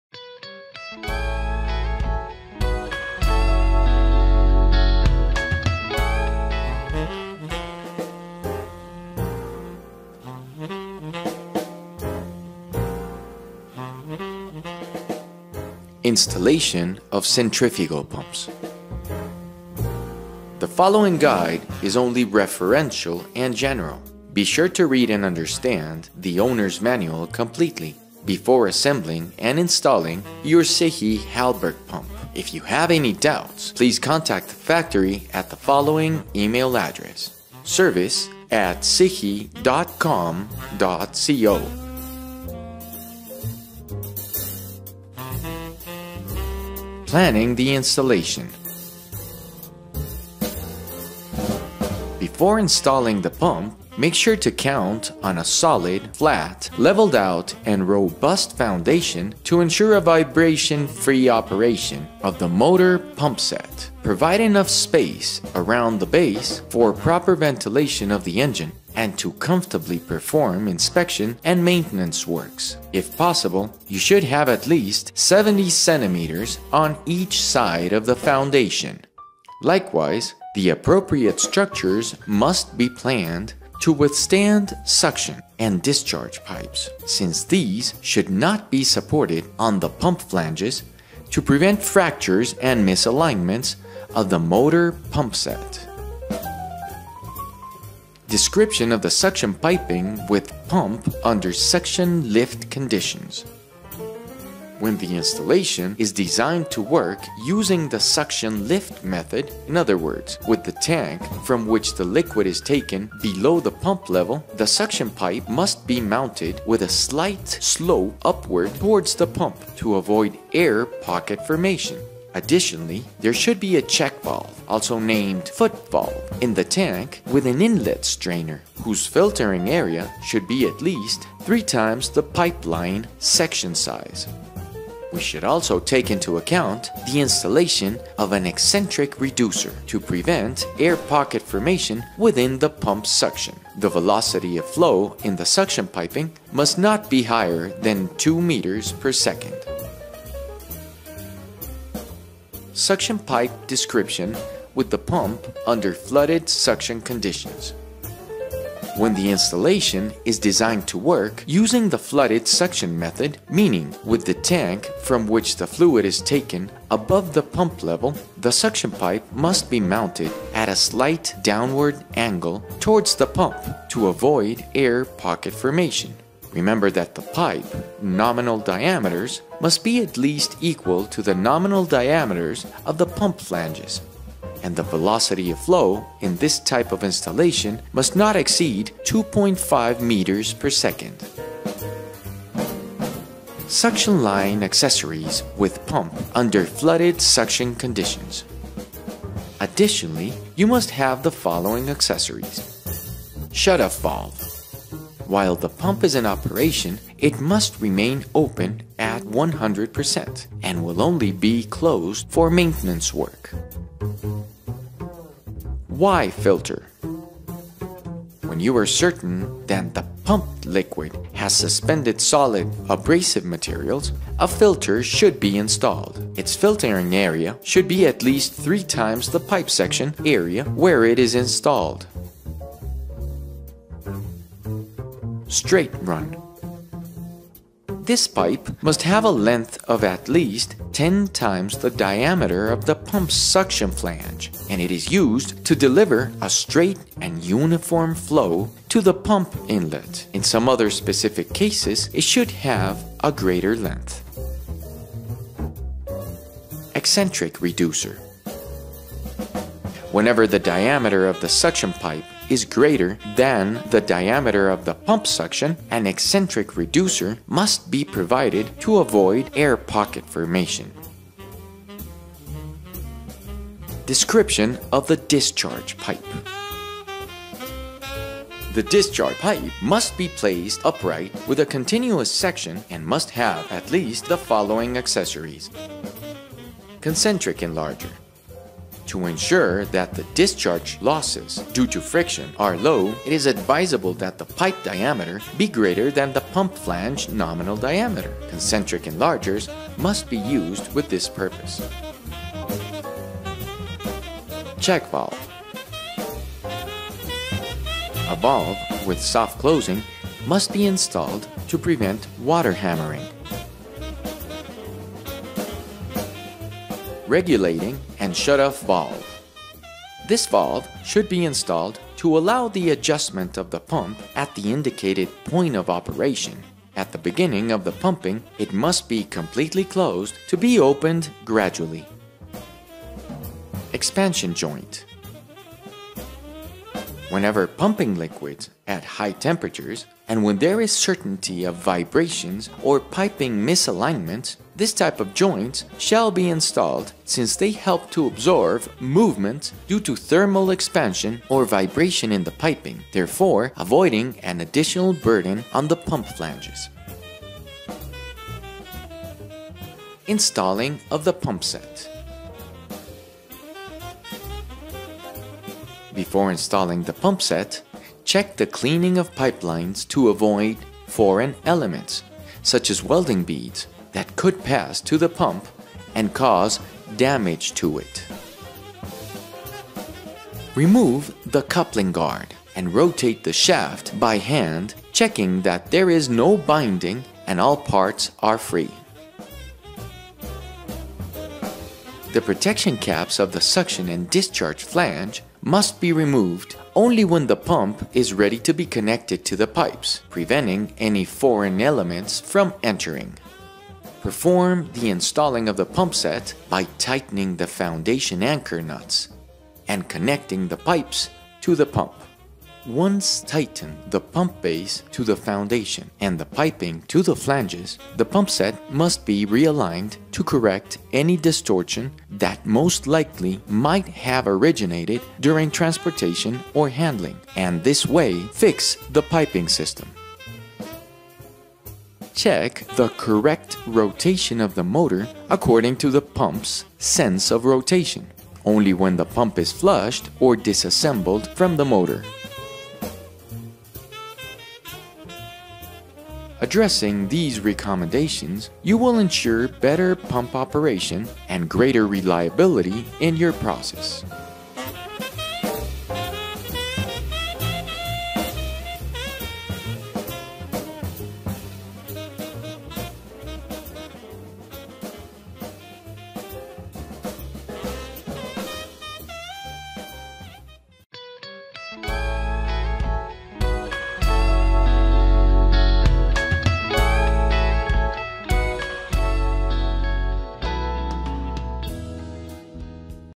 مترجم - شرح تركيب المضخات الطاردة المركزية Installation of Centrifugal Pump
فيلم مترجم باللغة العربية عن تركيب المضخات الطاردة المركزية ، يتضمن الأساسيات الهامة للتركيبات لضمان أفضل تشغيل للطلمبة..
يعرض الفيديو نصائح التركيب في حالة كون مستوى السائل أقل من مستوى المضخة وأيضاً في حالة كونه أعلى من مستوى الطلمبة وكذلك الوصلات الصحيحة اللازم تركيبها لكل من خط السحب وخط الطرد في الحالتين.
محتويات الفيديو:
- التخطيط للتركيب.
- وصف أنبوب السحب لمضخة تحت شرط السحب الصاعد.
- وصف أنبوب السحب لمضخة تحت شرط السحب المغمور.
- الملحقات الاضافية لخط السحب.
- وصف انبوب الطرد (التصريف).
- تركيب مجموعة الضخ (الطلمبة+الموتور).

"الموقع العربي الأول للمضخات الطاردة المركزية"
يمكنك زيارة موقعنا لمتابعة العديد من الشروحات المصورة والمكتوبة كما يمكنك الحصول على الدعم الفني.

Arabic video about Installation of centrifugal pump .. there are more important recommendations to ensure better pump operation
Planning the installation
Description of the suction piping with pump under suction lift conditions
Description of the suction piping with pump under flooded  suction conditions
Suction line accessories with pump under flooded suction conditions
Description of the discharge pipe
Installing of the pump set

The first Arab site for centrifugal pumps